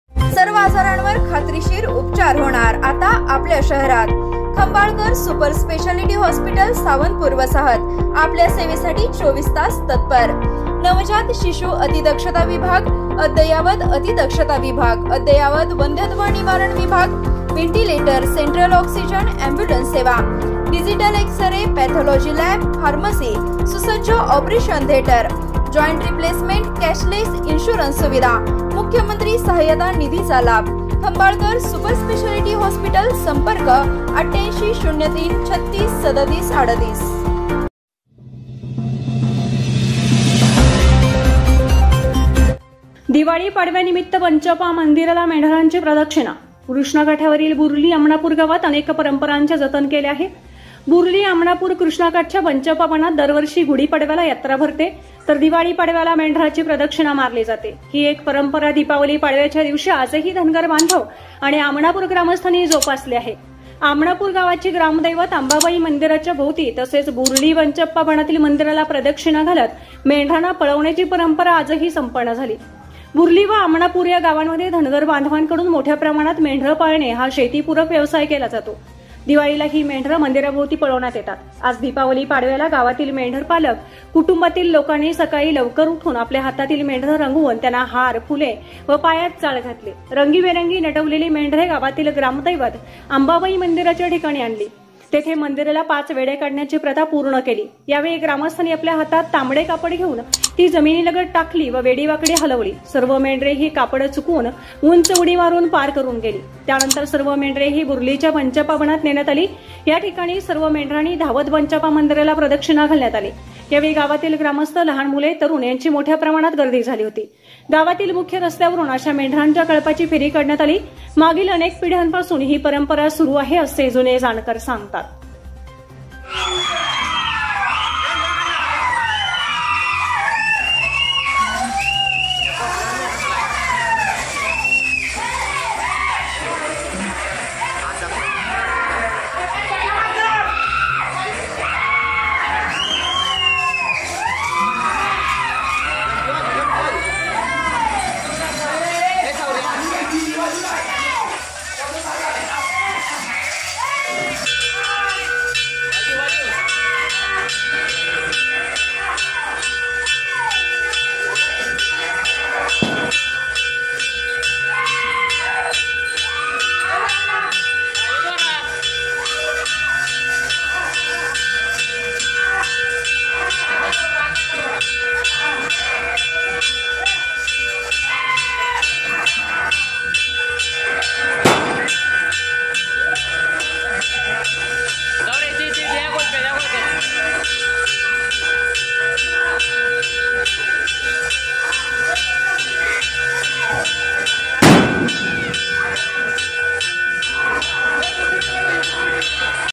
दिवाळी पाडव्यानिमित बंचाप्पा मंदिराला मेंढरांची प्रदक्षिणा
पलूस न्यूज एक्स्प्रेसचे व्हाट्सअप चॅनेल